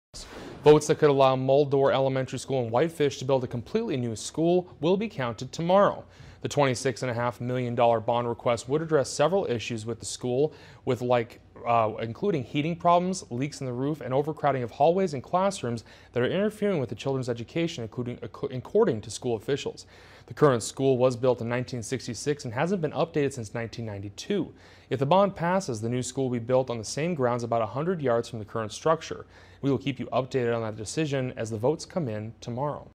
Fate of new Whitefish elementary school being decided
The votes that could allow Muldown Elementary School in Whitefish to build a completely new school will be counted on Tuesday.